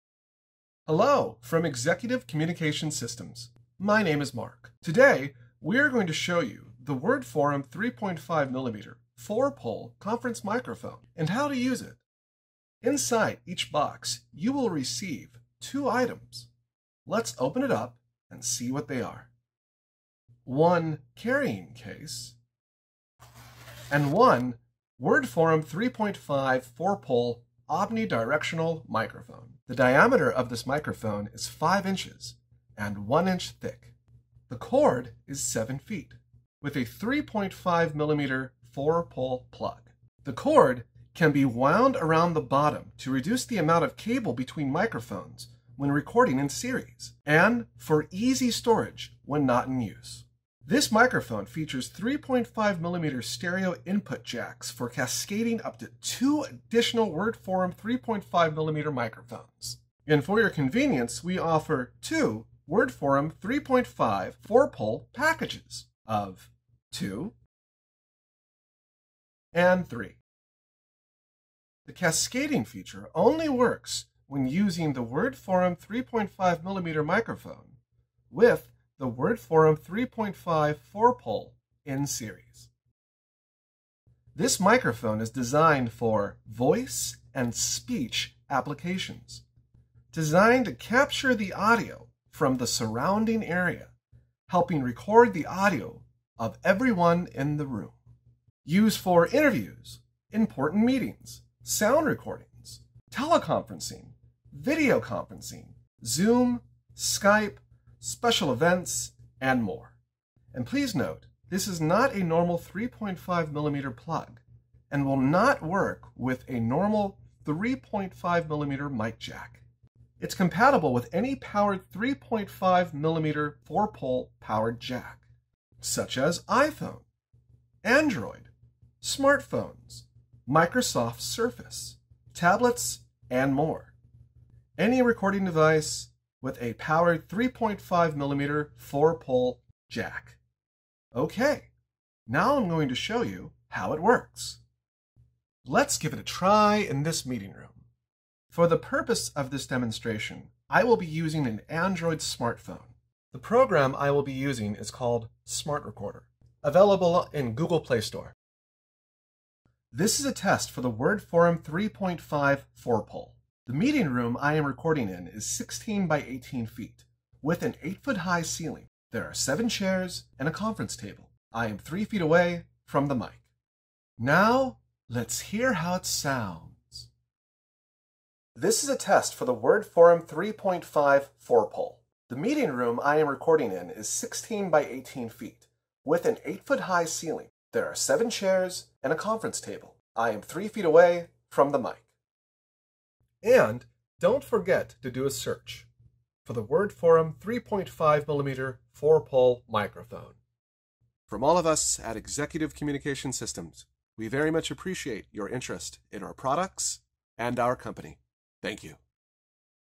WordForum 3.5 mm 4 pole 360° Omni-directional Conference Recording Microphone
ECS-WFCM3.5-4P WordForum 3.5 mm 4 pole 360° Omni-directional Conference Recording Microphone for Meetings using iPhone, Android, Smartphone, Microsoft Surface and other tablets with 4 Pole with Daisy Chain Option